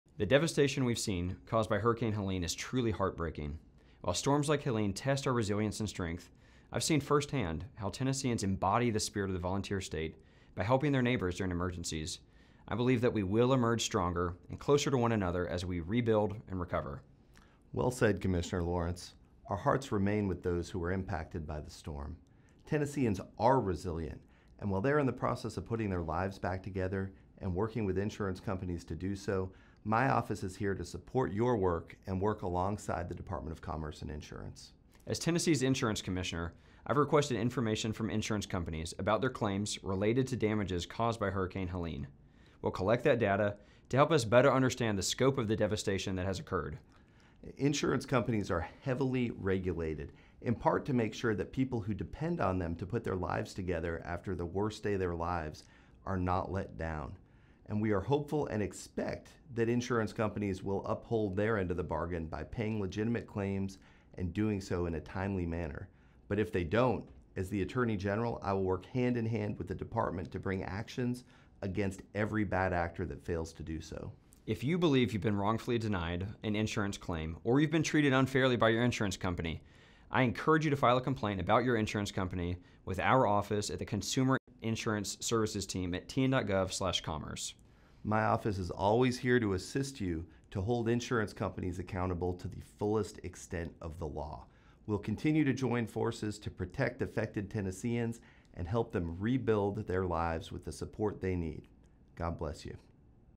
TDCI, Tennessee Attorney General Collaborate to Assist Tennesseans Affected by Hurricane Helene
Tennessee Department of Commerce & Insurance Commissioner Carter Lawrence and Tennessee Attorney General Jonathan Skrmetti are collaborating to help Tennesseans affected by Hurricane Helene by gathering crucial insurance data related to the storm while protecting consumers from bad actors.